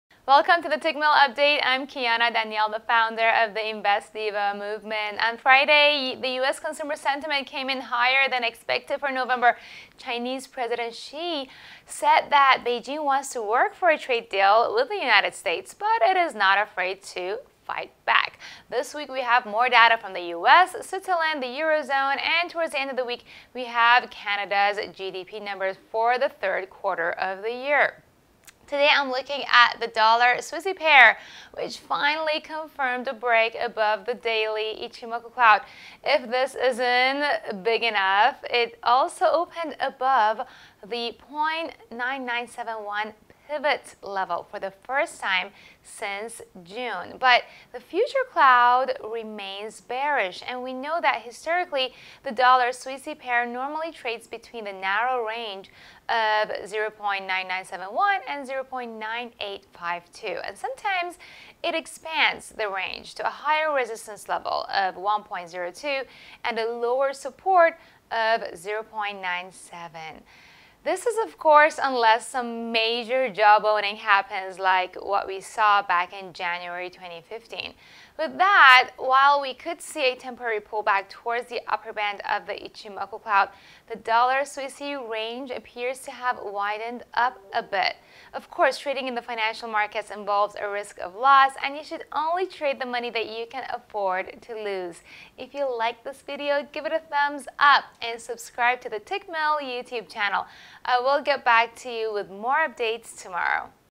November 25th, 2019 - USDCHF Breakdown
We're staring the trading week with an eye out for US, Swiss and Eurozone data with Canada's Q3 GDP figures coming up as the week ends. Meanwhile our Investing Diva will be examining the USDCHF's market movement!